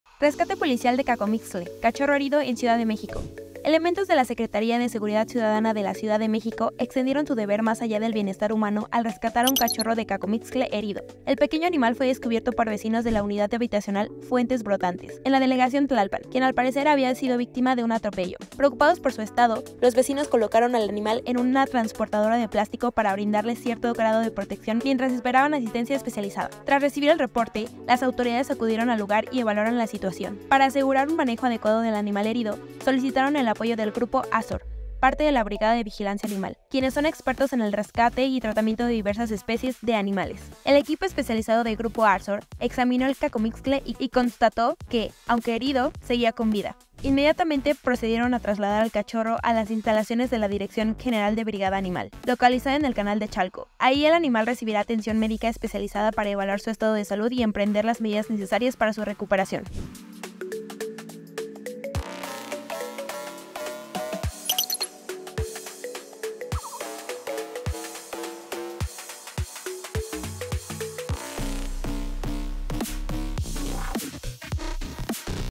Policías de la SSCDMX ayudan a cachorro de cacomixtle atropellado
Vecinos de Fuentes Brotantes en Tlalpan encontraron a un Cacomixtle cachorro herido en medio de la avenida principal del conjunto habitacional.